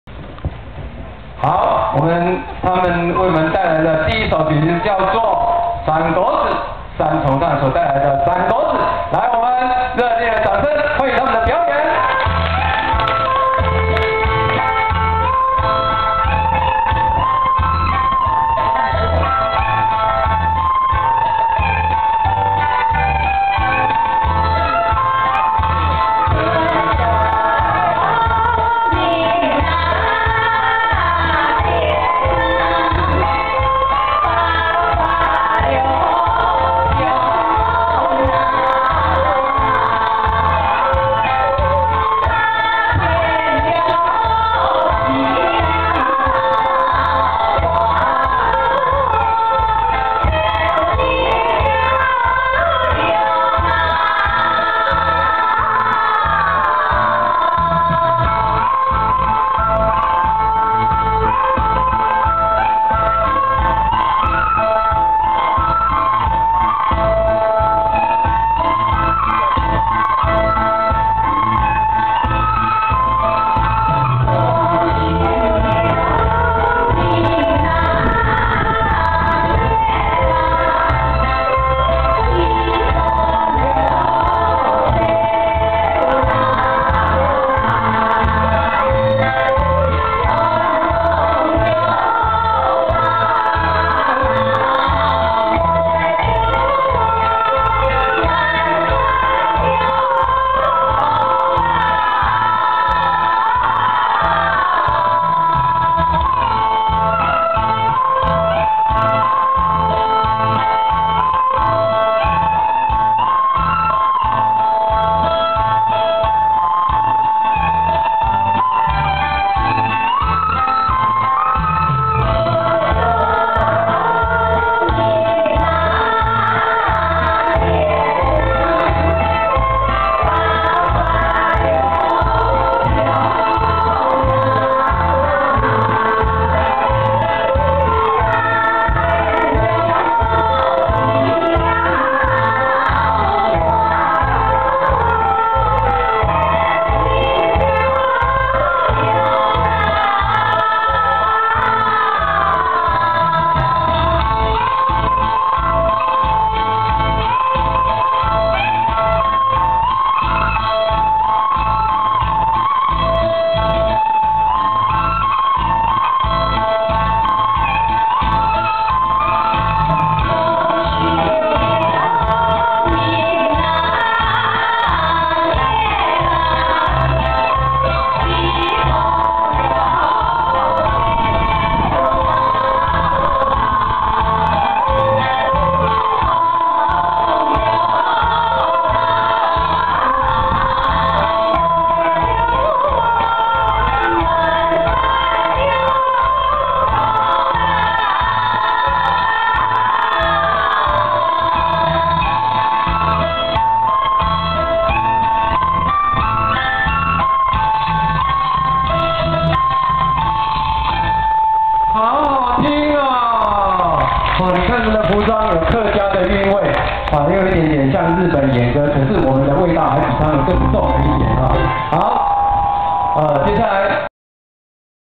2009星光燦爛 桃源幼鐸獎 山歌仔「三部重唱」
2009星光燦爛 桃源幼鐸獎 山歌仔「三部重唱」
演唱者：葉美月、陳秀珠、杜安昭